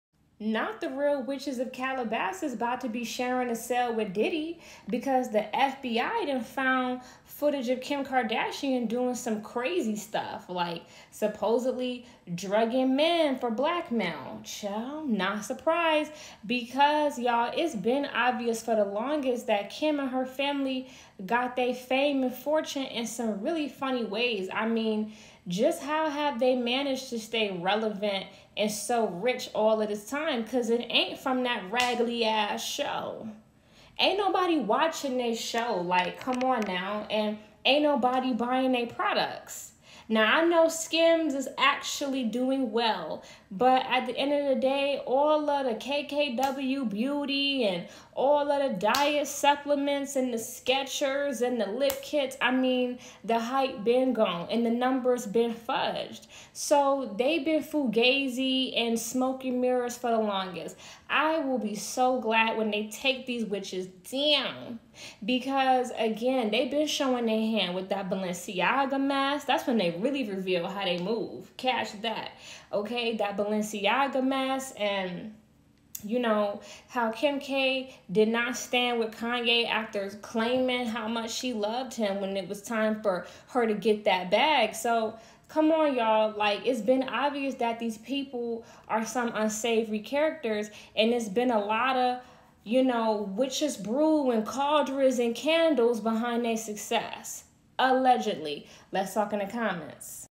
Kim Kardashian Exposed By the Feds In Diddy House Raid, The Kardashian Reign is OVER
Kim Kardashian done fled the country and is being Investigated by the FBI for Footage Obtained during diddy house raid kimkardashian the wicked witches of calabasas finally about to go down! they money and fame been fugazi for years! and her hulu show been cancelled, bye sis

Welcome to Pop Culture Conspiracy, your #1 source for underground urban pop culture news and commentary